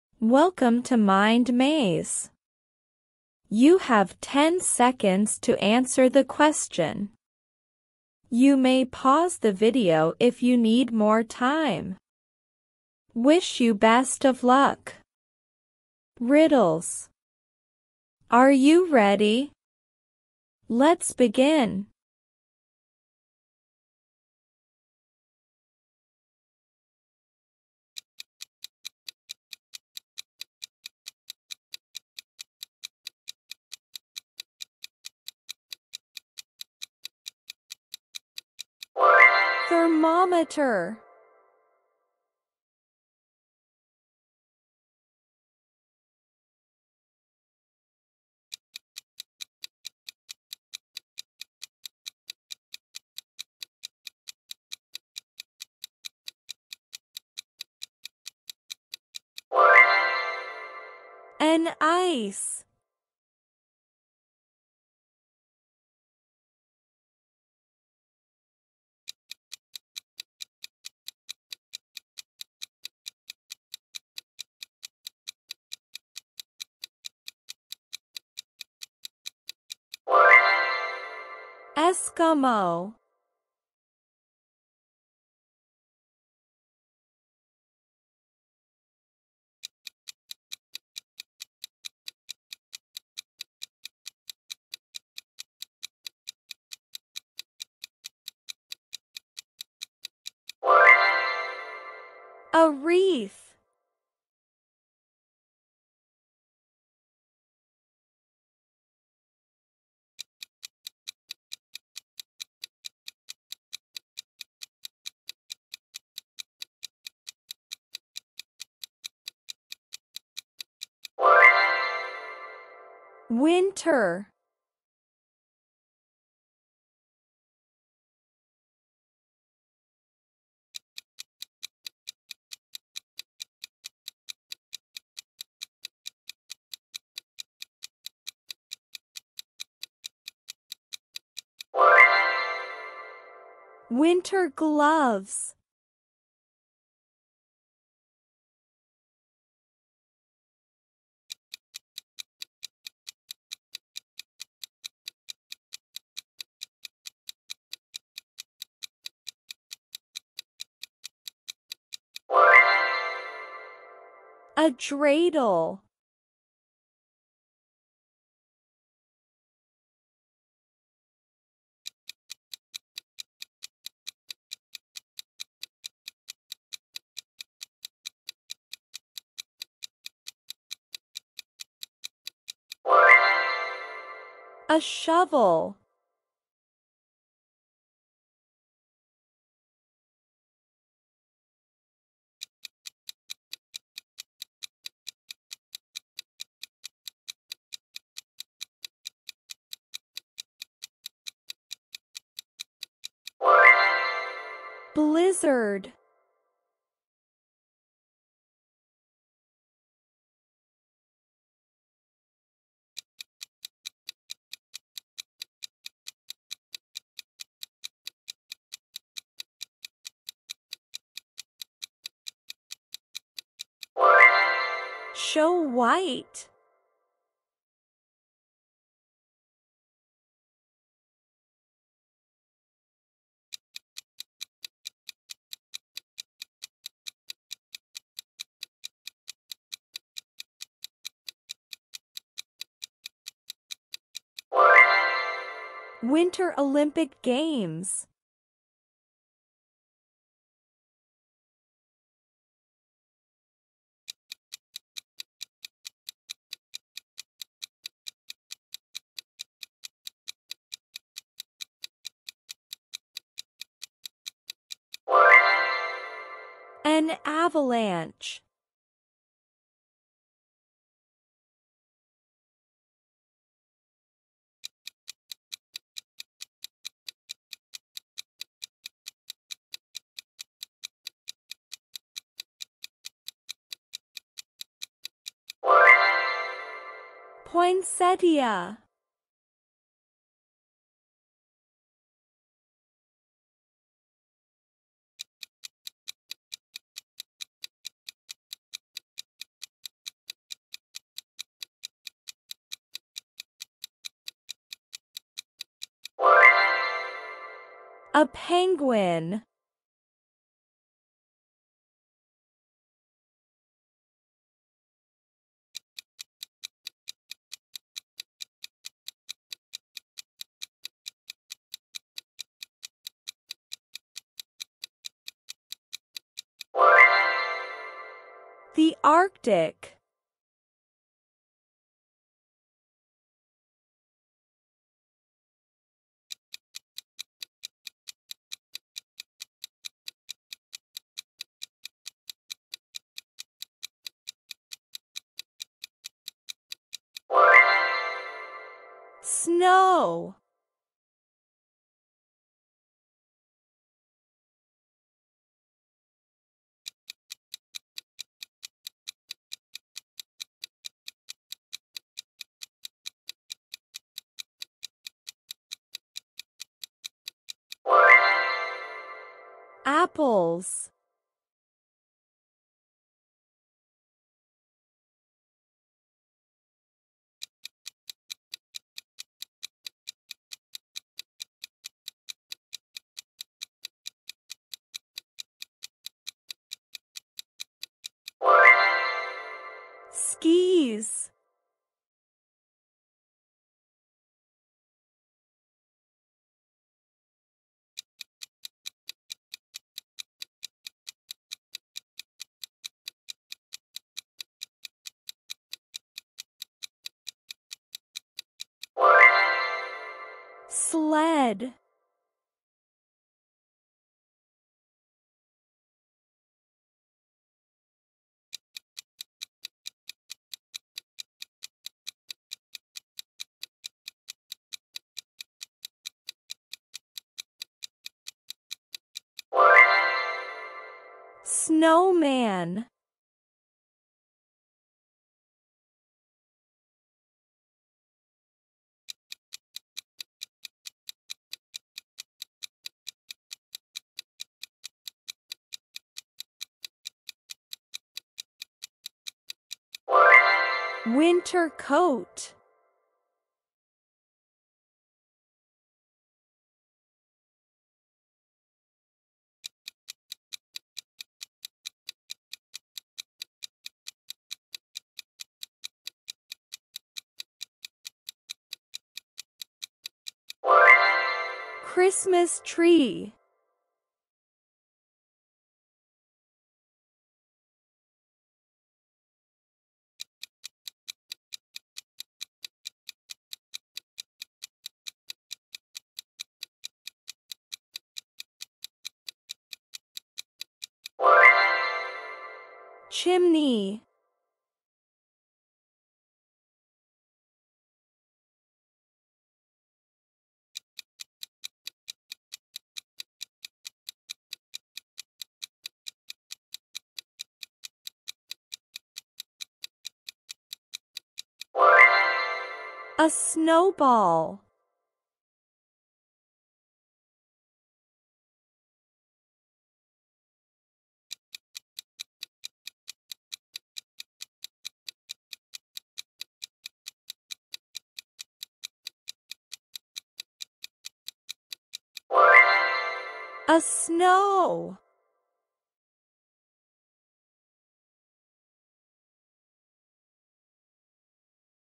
Frosty Puzzles: Wintertime Riddles to Baffle Your Brain! | Mind Maze
🌨️ Embrace the chill and test your wits with our Winter Riddles challenge! 🧩🌬️

Get ready to embark on a frosty brain teaser adventure that will keep your mind as sharp as icicles! In this exciting video, we've gathered a collection of captivating riddles that are as cool as the winter breeze. From snow-covered landscapes to snugly indoor scenes, these riddles will transport you to the heart of the winter season.

🔍 Put your detective skills to the test as you decipher clever clues and work your way through each mind-bending puzzle. Whether you're snuggled up by the fireplace or enjoying a snowy day, these riddles are the perfect companions to keep you entertained and engaged.

❄️ Challenge yourself and your friends to see who can solve these riddles the fastest! Share your answers in the comments below and join the frosty fun community of fellow riddle enthusiasts.

🎉 Don't miss out on the brain-teasing excitement! And remember, the cold might be icy, but your logic and creativity can melt away even the toughest riddles!

Get ready to bundle up with a warm cup of cocoa and let the Winter Riddles challenge begin! ❄️🧠🔥

Thanks for Visiting @mindmaze40